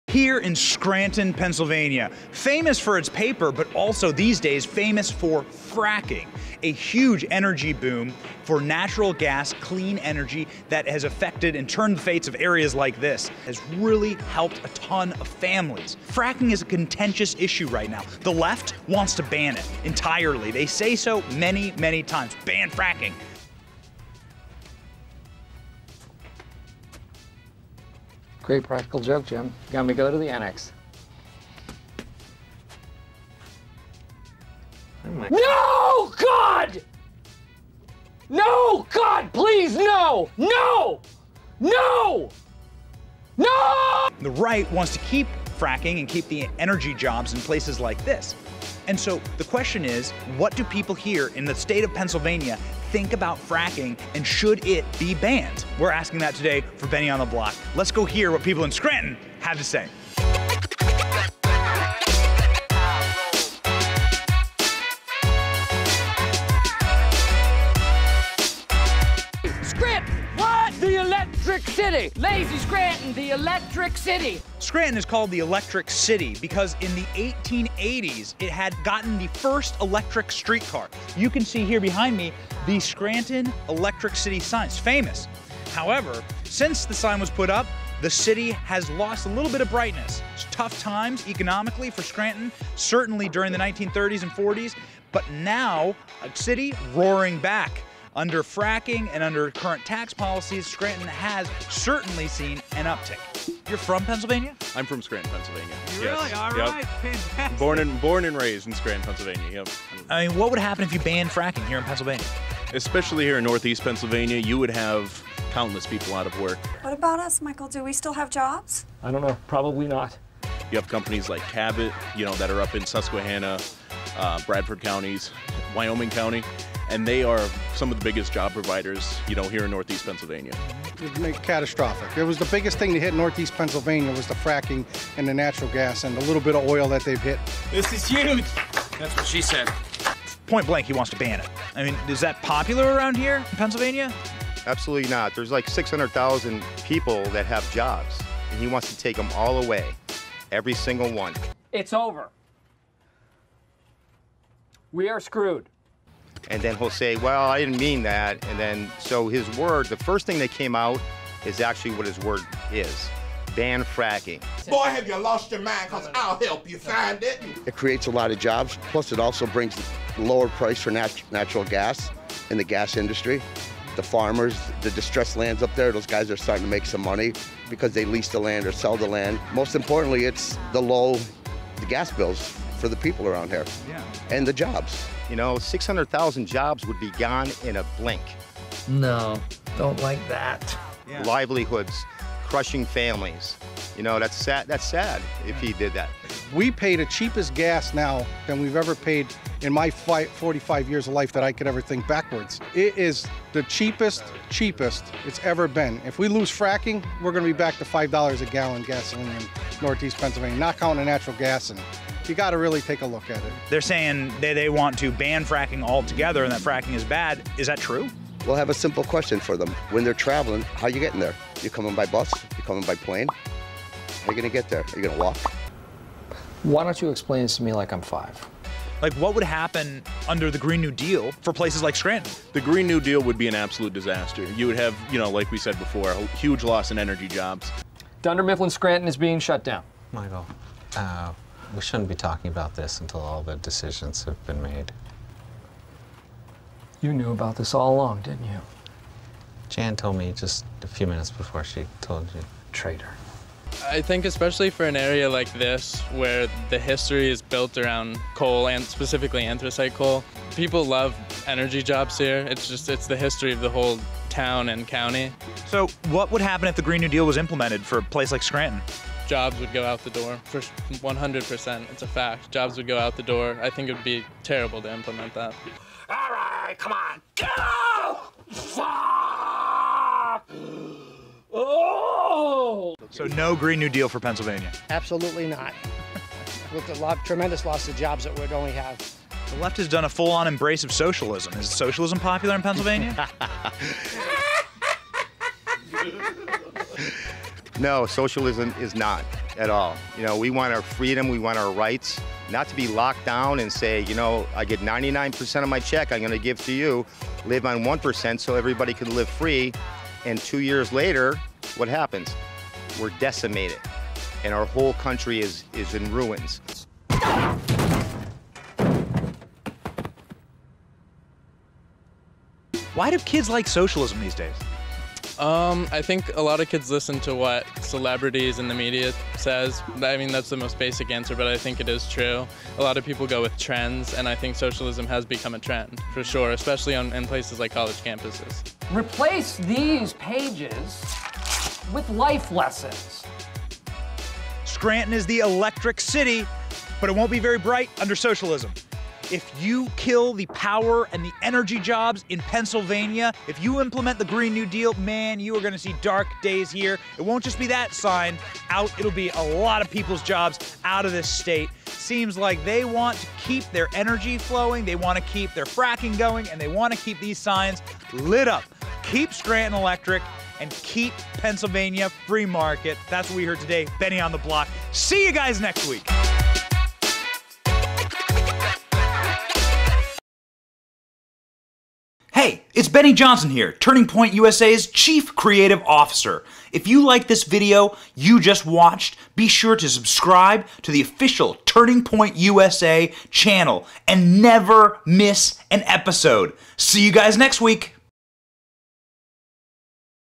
Ep 25 | Do People In Pennsylvania Want The Green New Deal?
This week Benny Johnson interviews people in Scranton, Pennsylvania. Scranton is called the electric city because in the 1880s it had gotten the first electric street car. These days it is also famous for its fracking--an industry that employs thousands of people in the area.

Fracking is a contentious issue right now. Leftists want to ban it ENTIRELY to pave the way for their socialist Green New Deal.

What do the people here in the state of Pennsylvania think about fracking and the Green New Deal? Do they want to keep the fracking industry in their state, or are they ready for a socialist overhaul?